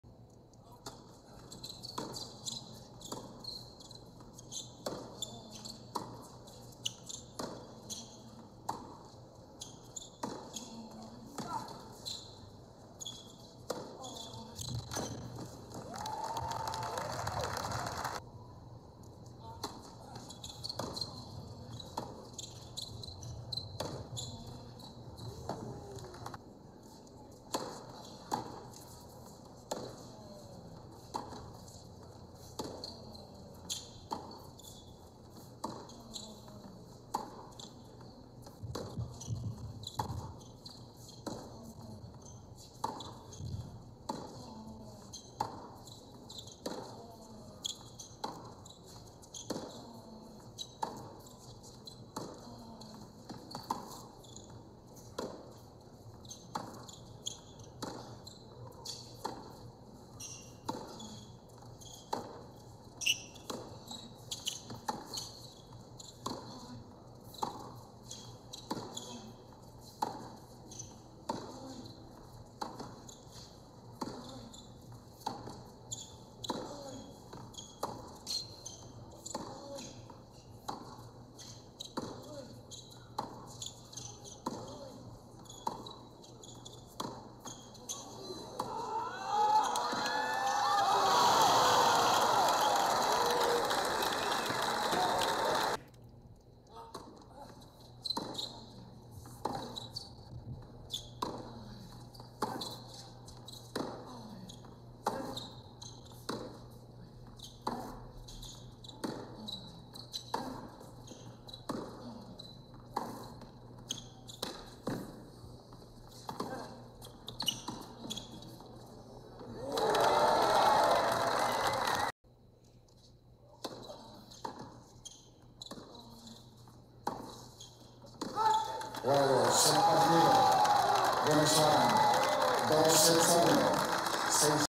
ATP Mexico: Prajnesh Gunneswaran vs Go Soeda - HLs
Prajnesh came back from a set down to complete a 3 set victory over Go Soeda in the semis of the ATP Challenger in Monterrey, Mexico

ATP Monterrey Challenger, Mexico

[R1] (7) Prajnesh Gunneswaran (IND,278) d. (WC) Ryan Harrison (USA,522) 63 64
[R2] (7) Prajnesh Gunneswaran (IND,278) d. Alex Rybakov (USA,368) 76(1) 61
[QF] (7) Prajnesh Gunneswaran (IND,278) d. (3) Jason Jung (TPE,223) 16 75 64
[SF] (7) Prajnesh Gunneswaran (IND,278) d. Go Soeda (JPN,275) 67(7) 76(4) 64
[F] (7) Prajnesh Gunneswaran (IND,278) vs. (1) Fernando Verdasco (ESP,150)